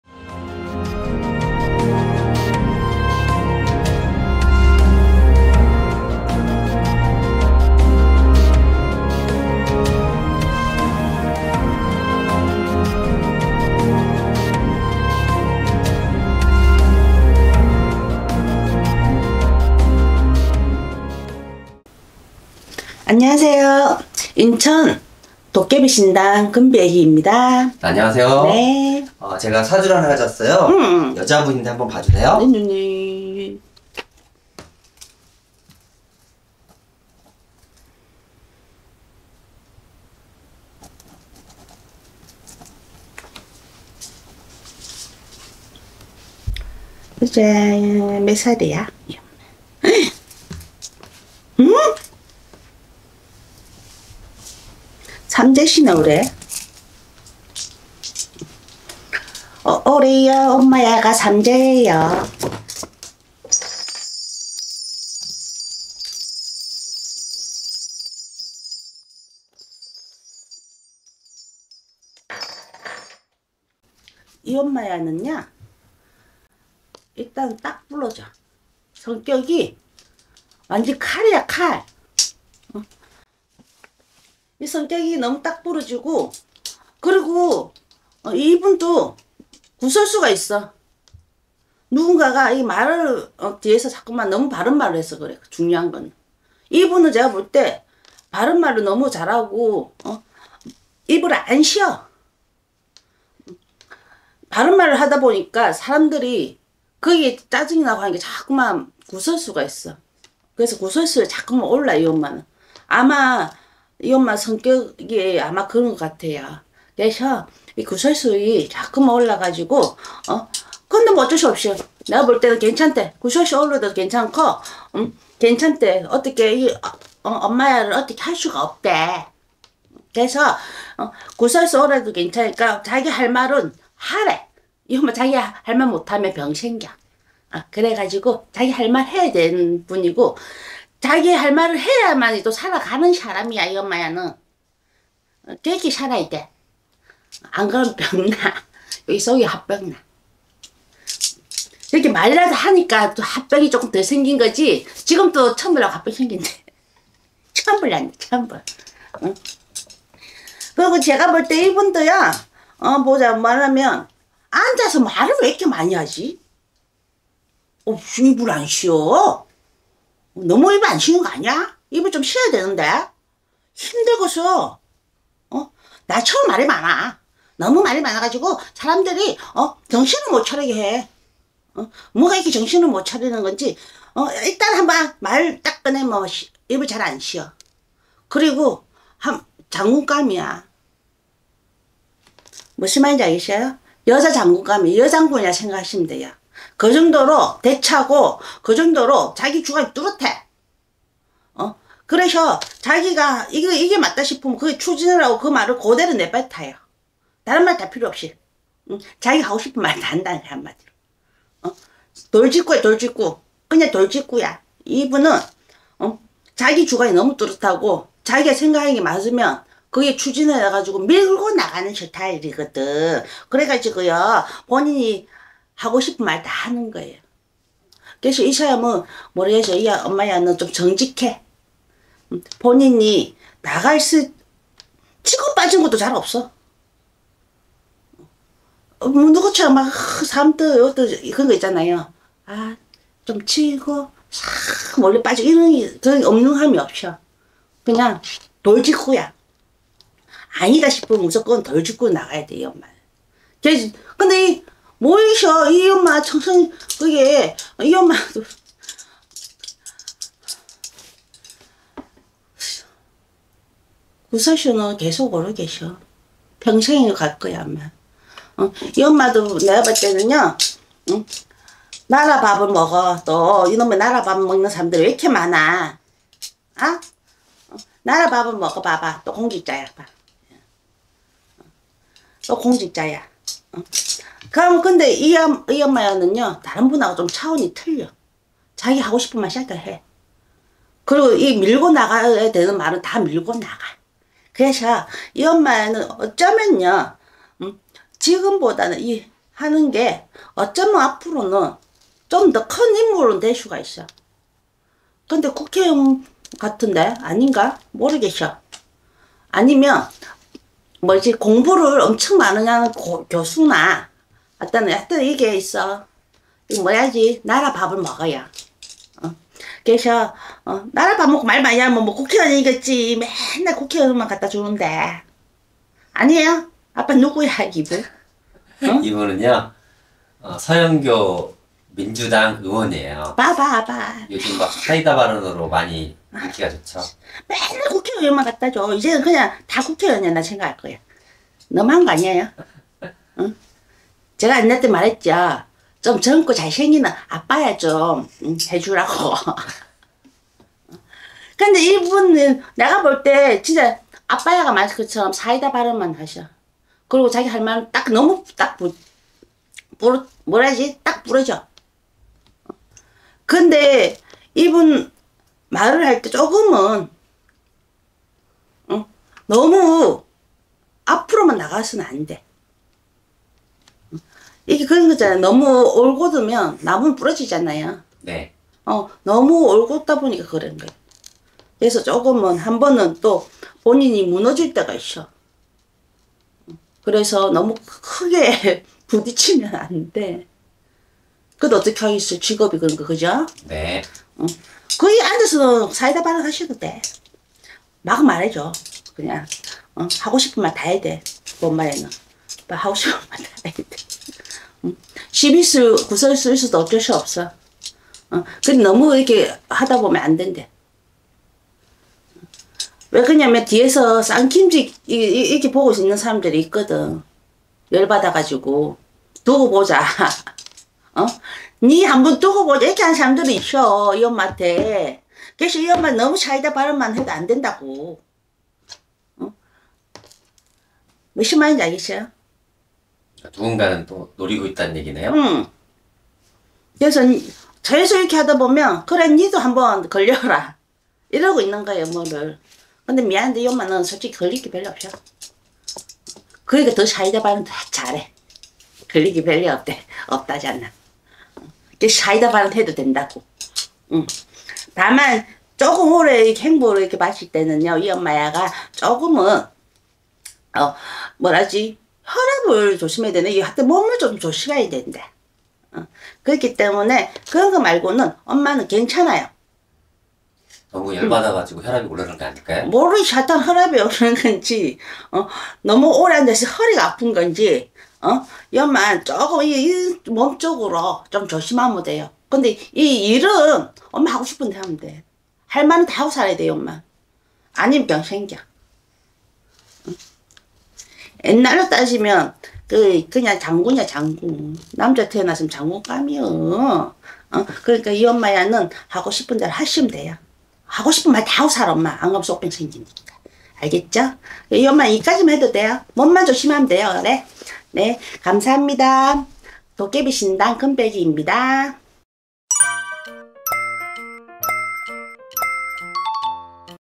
[천기누설]🌺"서영교"의원 블라인드 사주풀이🌺여장군감이다 💦인천 도깨비신당📞상담:010-8082-3599
🌺시비,구설수 따르나 괜찮으니 밀어 부쳐라🌺인천 도깨비신당 (구.연화당)📞상담:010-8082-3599

🔴【구독】【좋아요】【알림】 잊지마세요~!!🔴